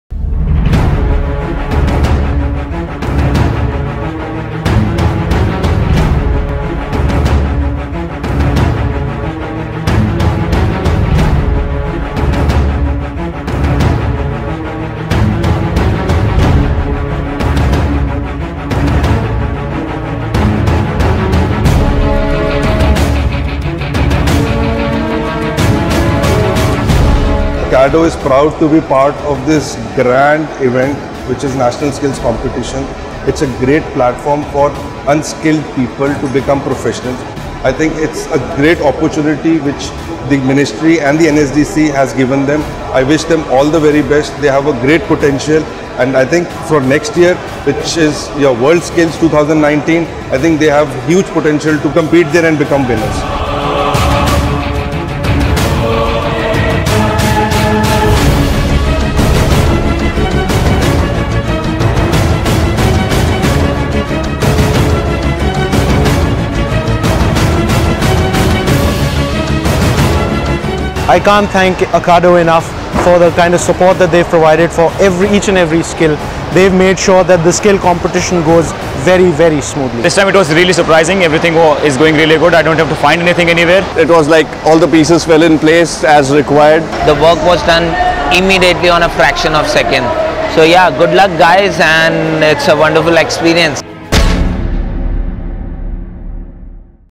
IndiaSkills 2018 - Powered by Akkado
India's biggest skill competition, Indiaskills 2018 brings a unique platform for the youth of India to showcase their skills and talent and be a part of India's successful and brighter future.